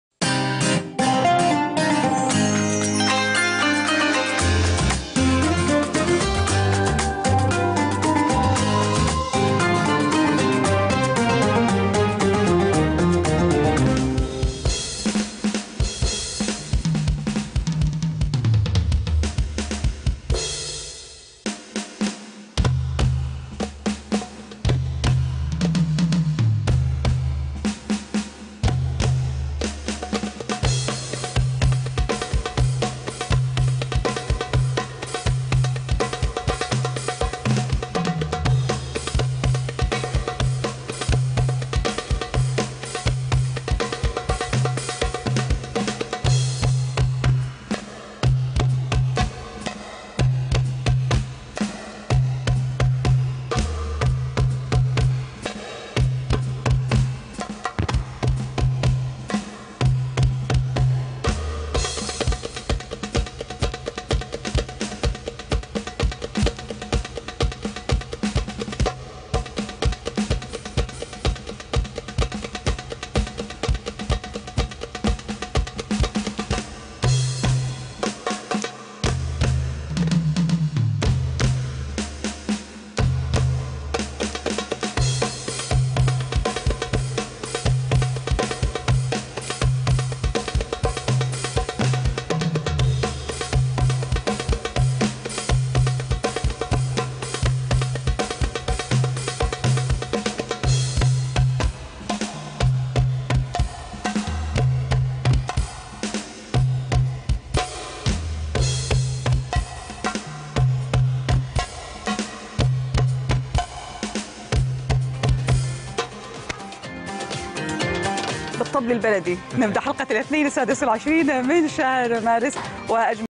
Wassim and Me
MBC 1 TV MORNING SHOW PERFORMANCE
Drum Duet
Wassim (Drum Student) on Hand Percussion Durbakke
Me on Electric Drums